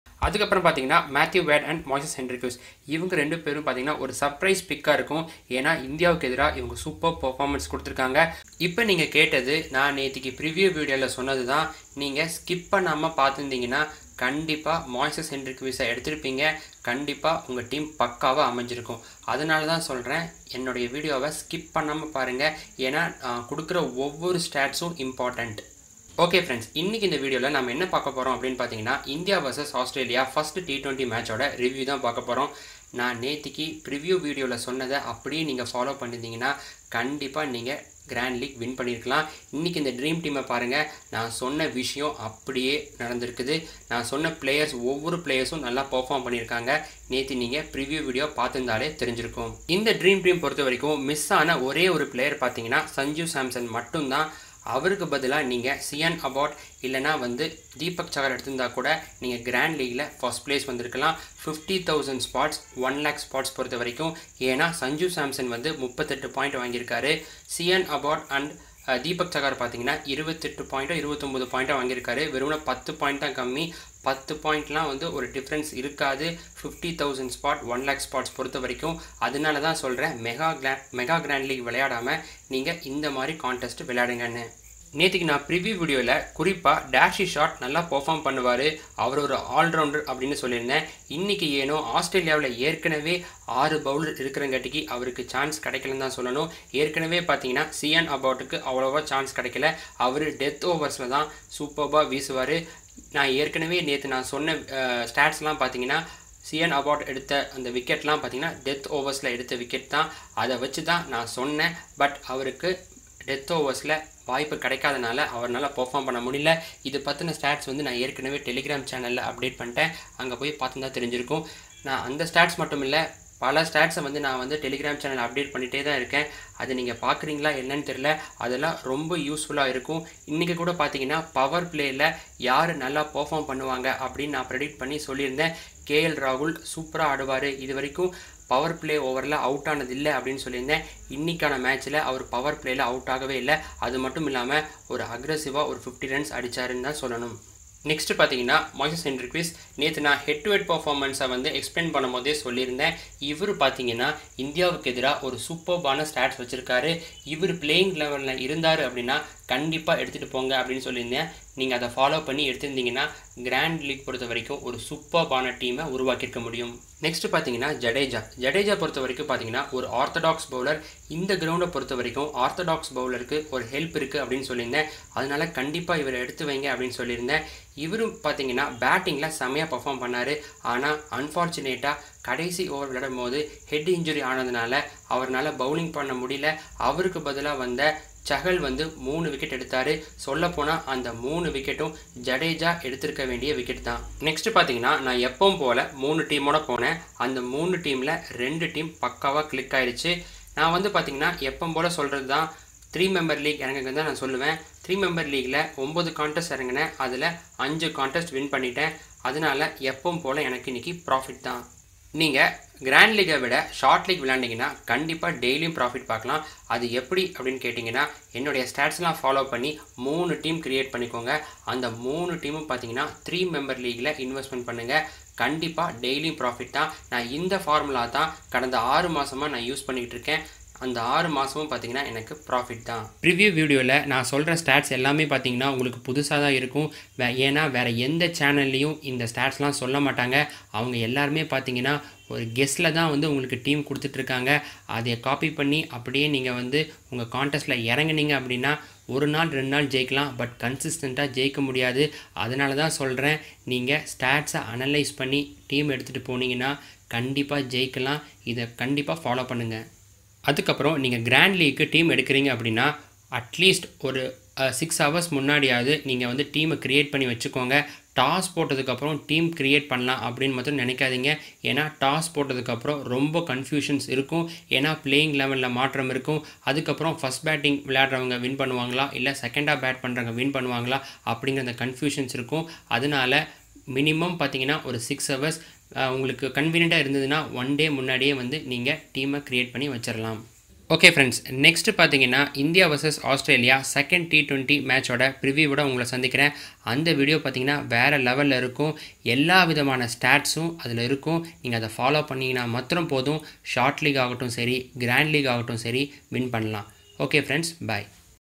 India Vs Australia 1st T20 Match Review | Our Prediction Success! | Fantasy Analyser
This Video is all about India Vs Australia 1st T20 Match Review